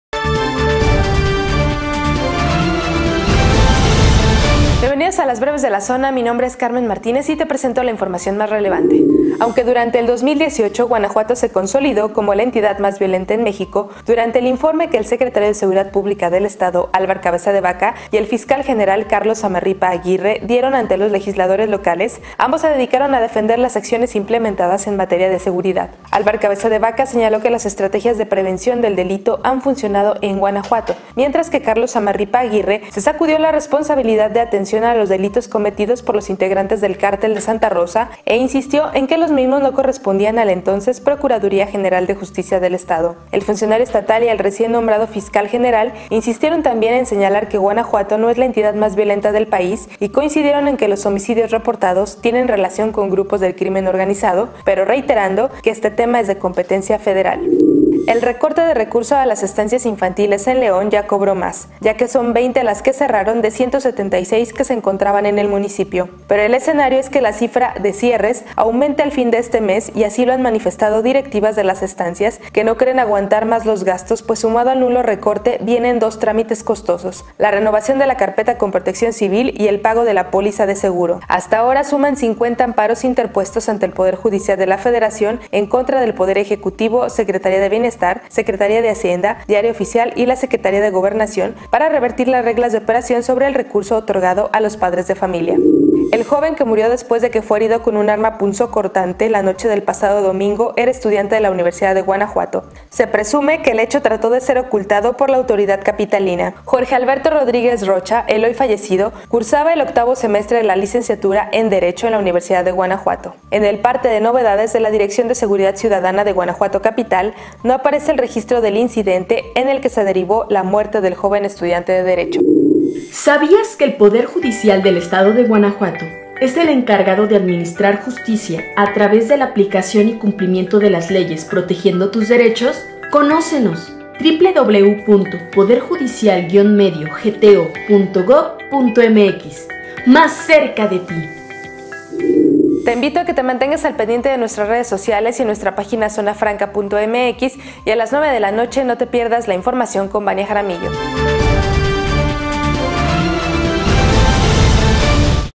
Breves de la Zona 20 de marzo 2019 emisión vespertina
Hoy en las BrevesDeLaZona con Carmen Martínez
- No somos el estado más inseguro, defienden Zamarripa y Alvar pese a incremento en la violencia en Guanajuato
- Suman 20 EstanciasInfantiles cerradas en León: piden préstamos y corren personal para aguantar.
- Joven asesinado en GuanajuatoCapital era estudiante de la UG; autoridad municipal no registró el hecho.

Estas y más noticias las encuentras en zonafranca.mx